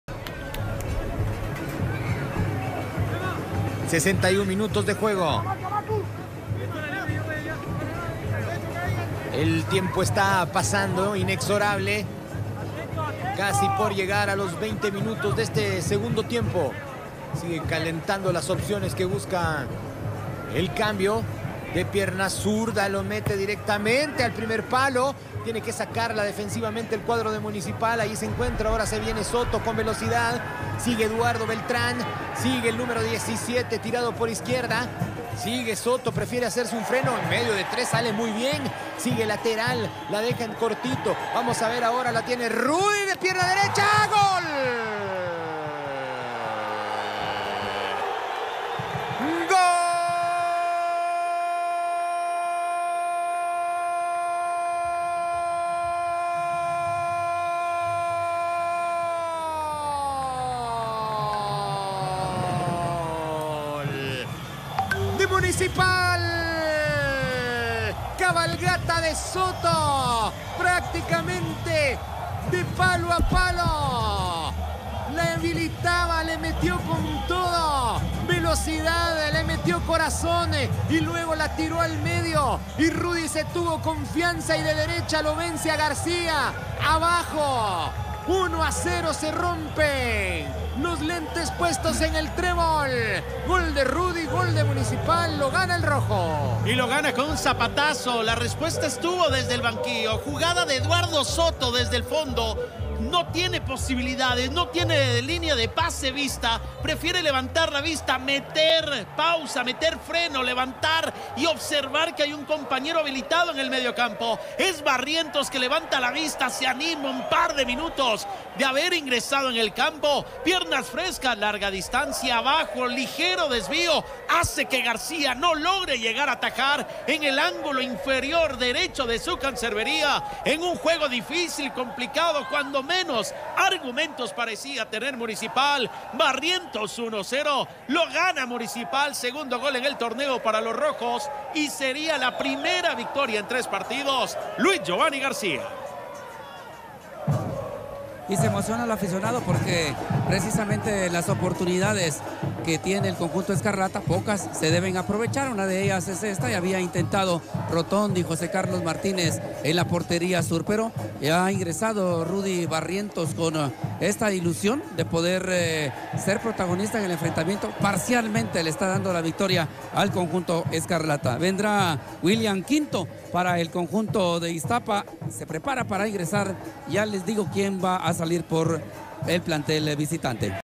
Gol de Municipal en el minuto 61 del partido
Anotación de RudyBarrientos para Municipal en el minuto 61 del partido ante Iztapa.